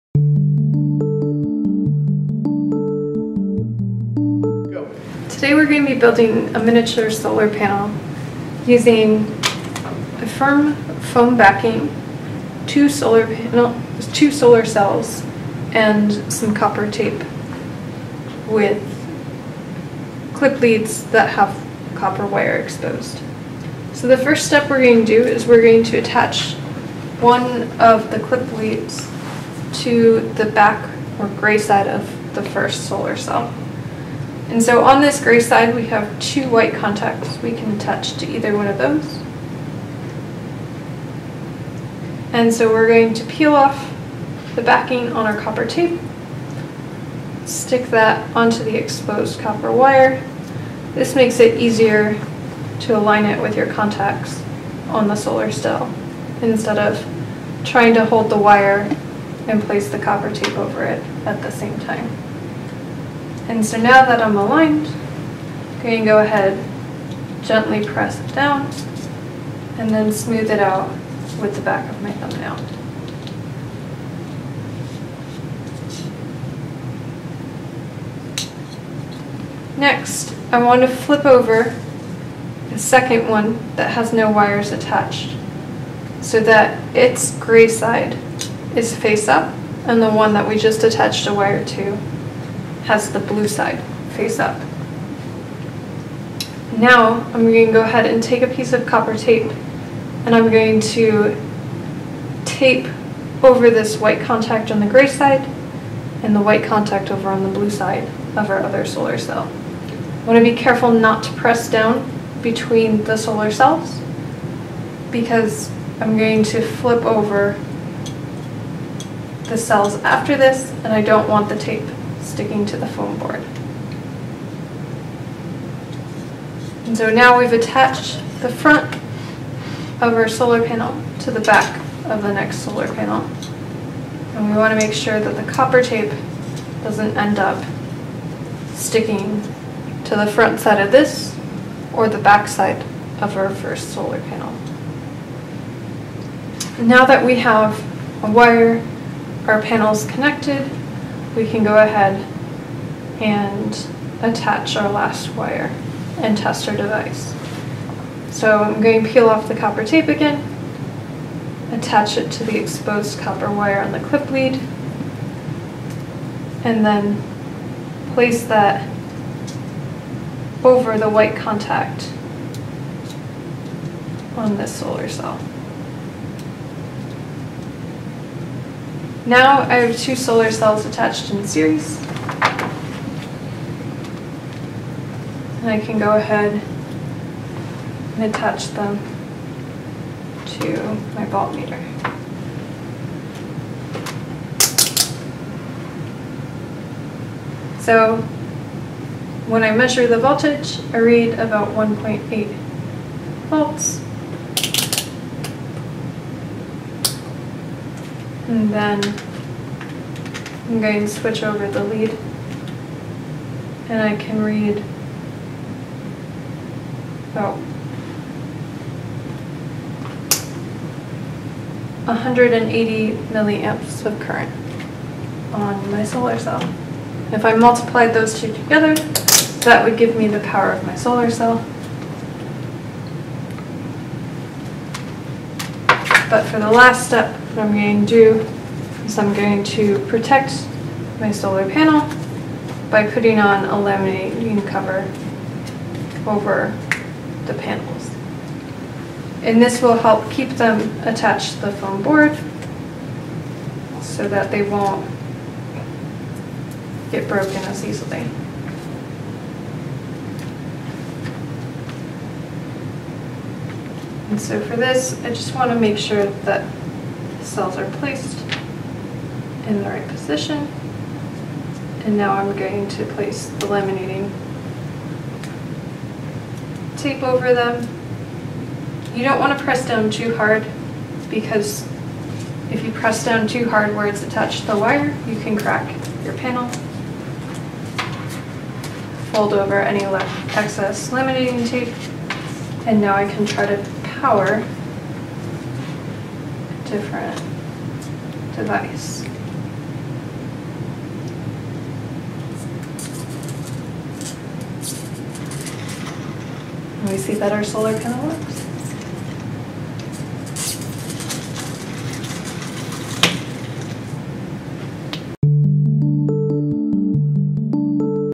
Make a mini solar panel for kids
Erin Jedlicka shows how to make a mini solar panel using solar cells, clip leads, copper tape and laminating film.